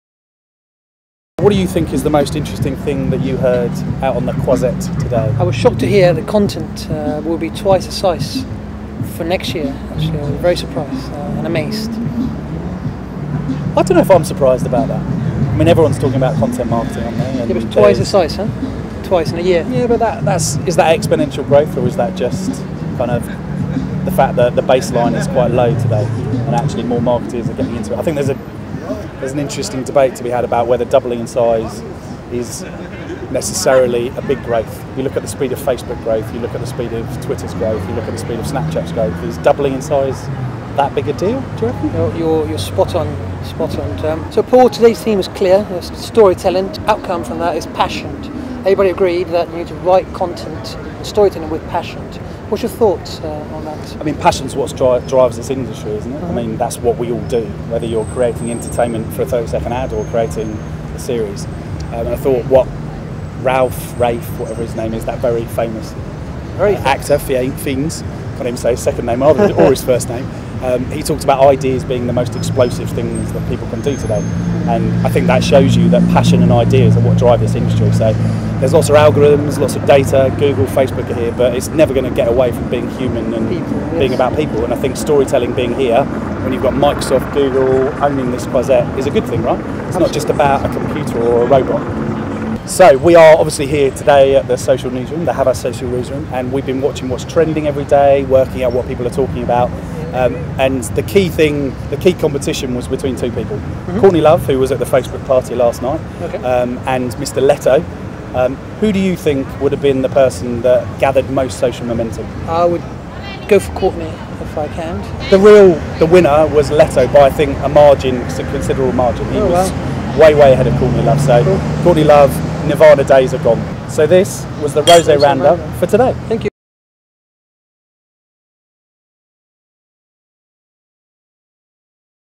HavasCafe Social Newsroom Day 3 Rosé Roundup
Paul Frampton and José Gutiérrez Cuéllar roundup our third day at CannesLions over a glass of rosé outside the HavasCafe Social Newsroom